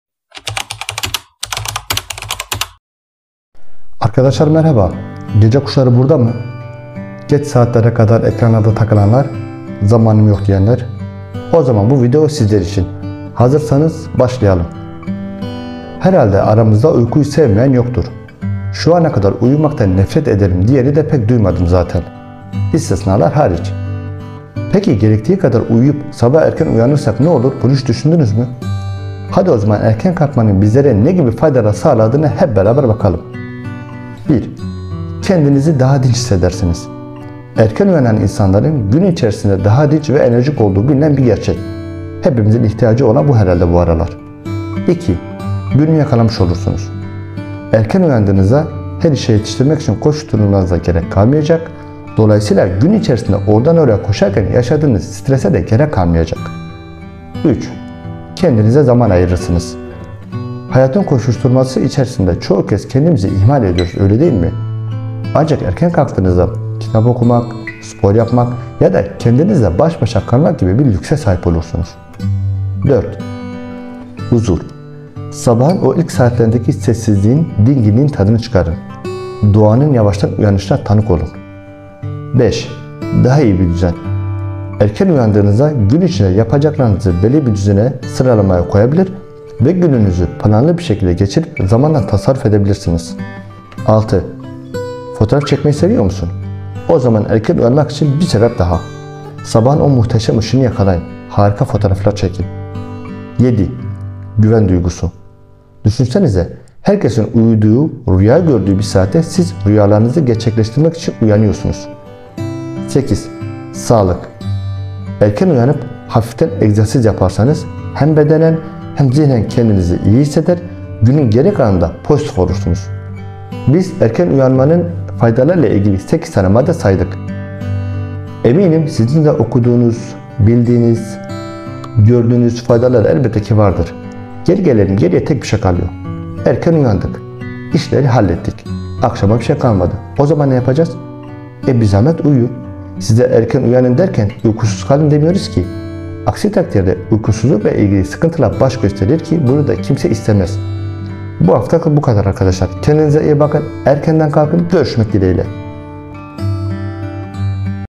sabah erken uyanmanın faydaları👍
sabah erken kalkmanin faydalari neler,nasil degerlendirebiliriz bu konuda kisacik bir video hazirladik..iyi seyirler🙂🙂